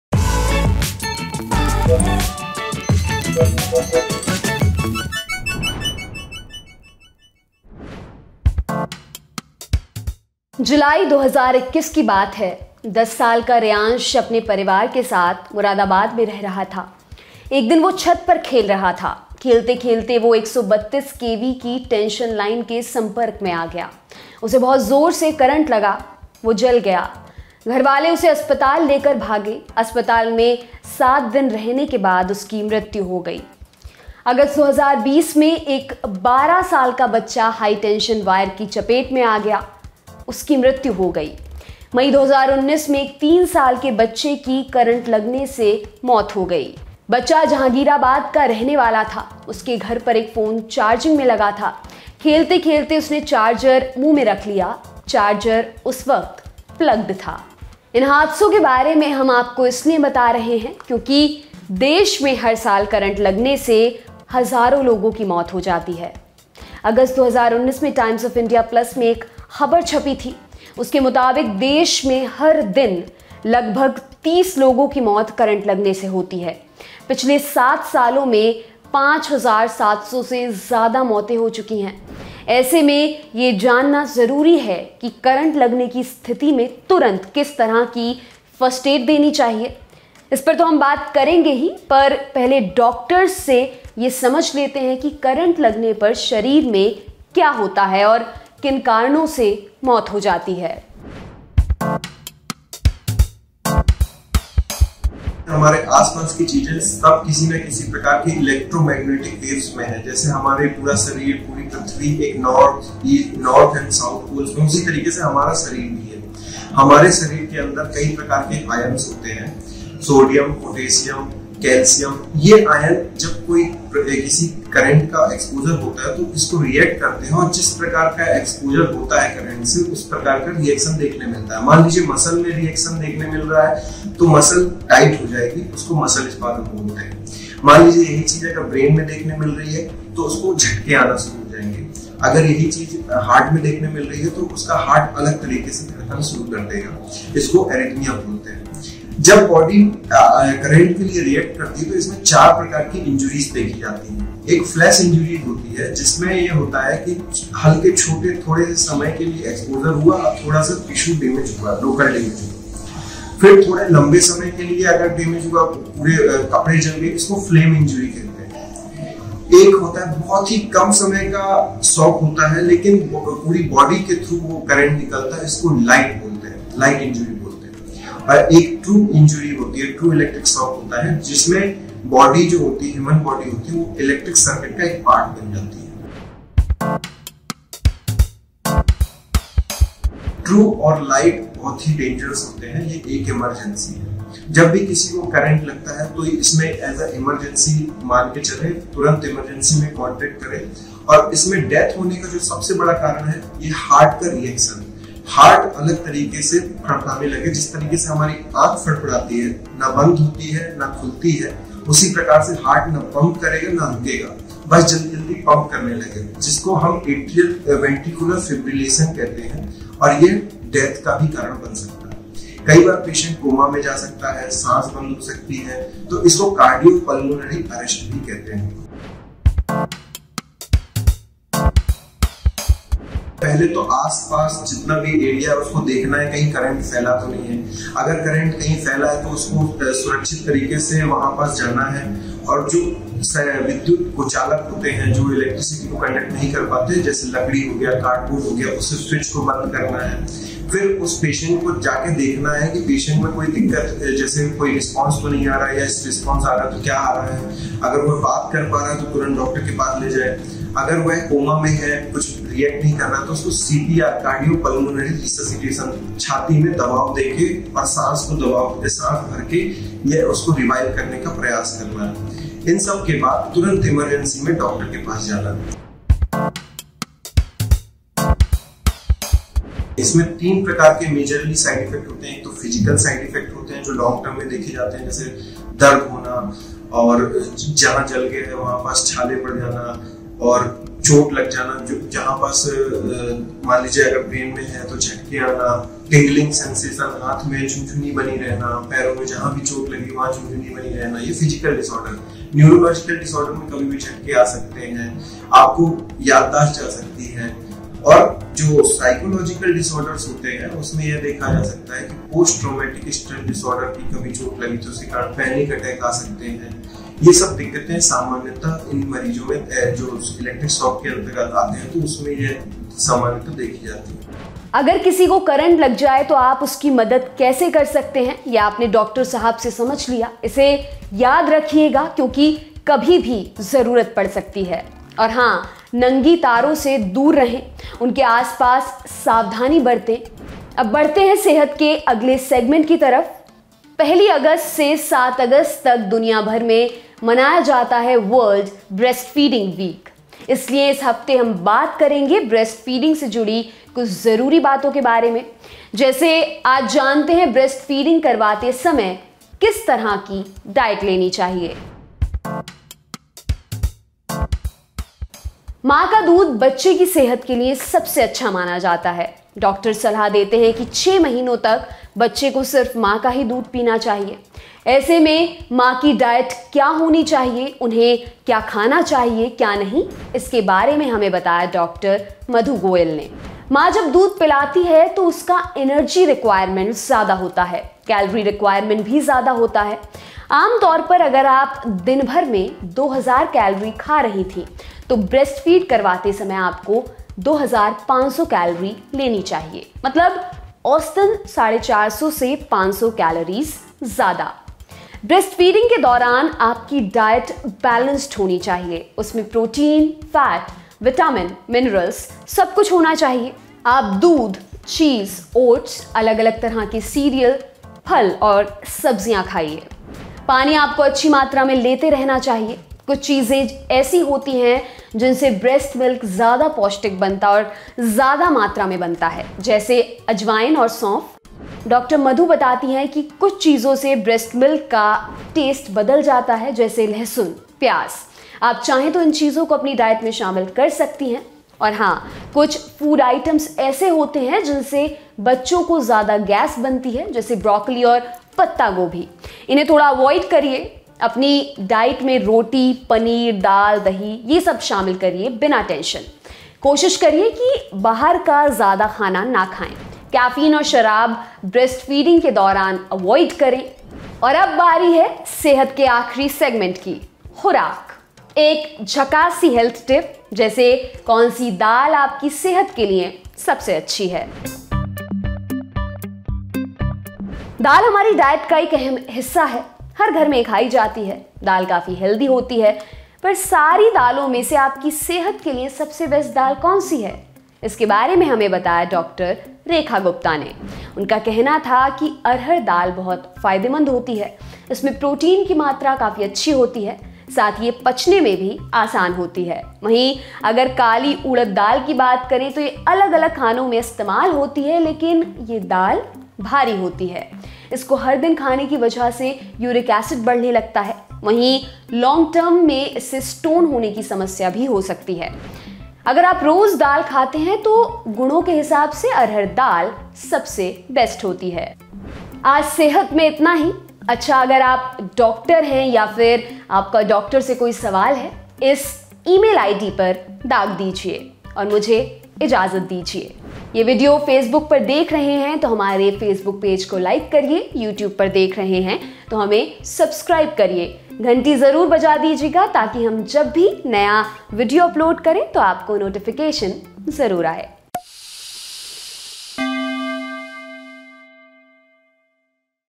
Electric Current लगने पर शरीर में क्या होता है, जानिए Doctors से | Sehat ep 224
Welcome to The Lallantop's  episode of Sehat. Your daily health show. In today's episode:  Electric shock is a common phenomena but most of us don’t know why a few second’s electric shock causes death. To discuss everything about electric shock we contacted critical care expert Dr Deepak Shukla , if you too want to know about electric shock from reasons to treatment and precaution, do watch. In next segment of Tan ki Baat, we will share the diet plan for lactating mothers. Last but not the least, find out which dal is the healthiest. Watch the video for details.